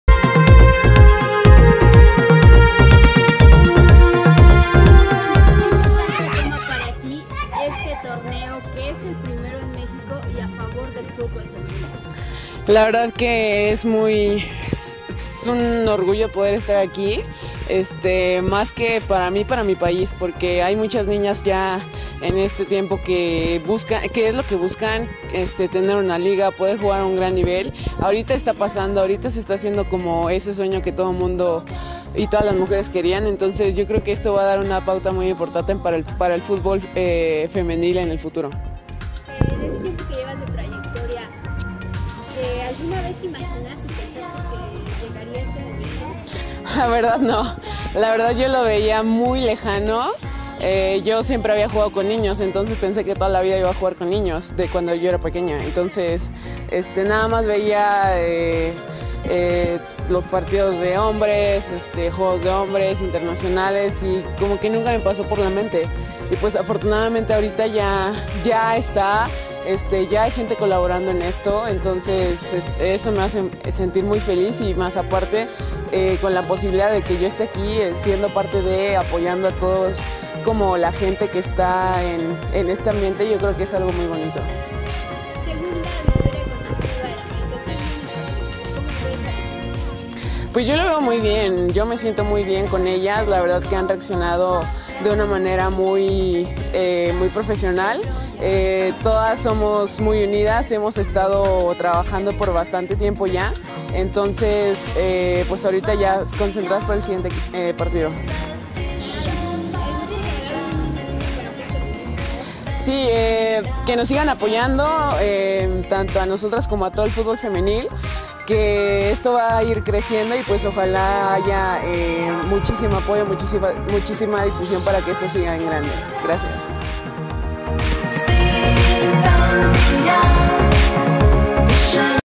Ceci Santiago - Portera del Club América y Seleccionada Nacional - Copa Mx Femenil
¿Pensarías que llegaría este momento? Checa aquí la respuesta de Ceci referente a la Liga Mx Femenil